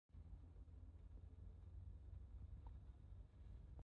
green anole on the move
The waterfall at Bethel Springs had a fantastic rainbow! Sunday March 26th 2023